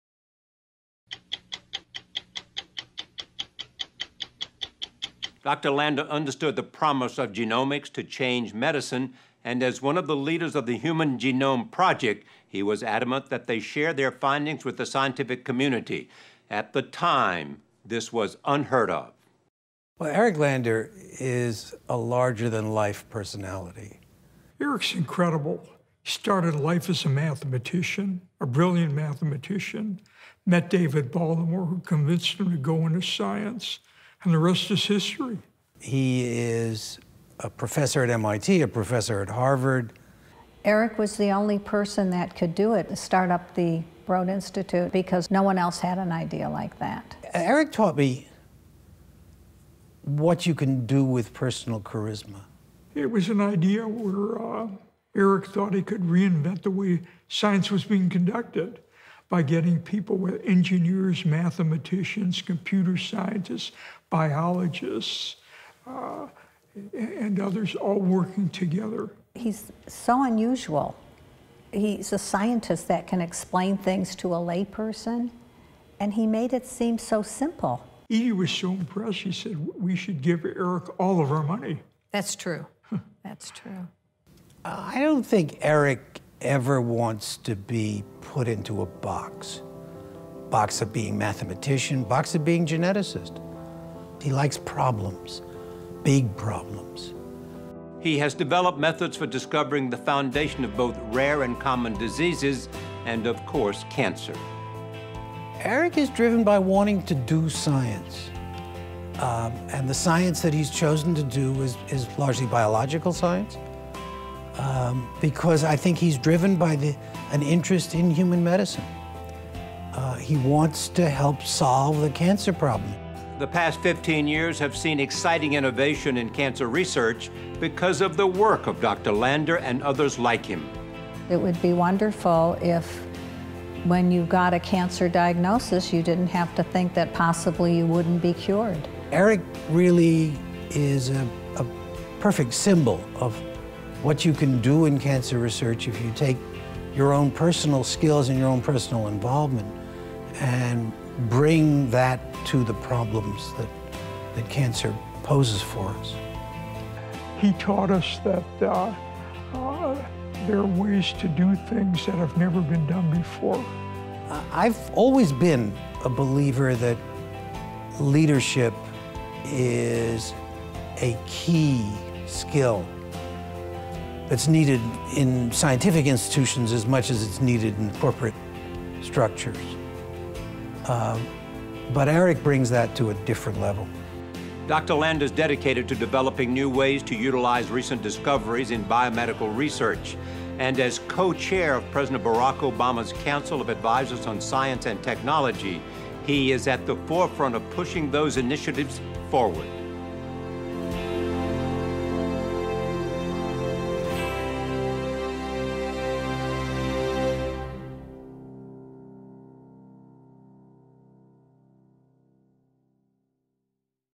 Friends of Cancer Research 20th Anniversary - Cancer Leadership Award
Dr. Eric Lander received a Friends of Cancer Research Cancer Leadership Awards for his effort and contribution in the fight against cancer.

Dr. Lander was honored for his work decoding a molecular taxonomy for cancers, which has allowed for an increased understanding of the molecular origins of these cancers. These taxonomies and increased understanding have in turn aided the design of more effective therapies.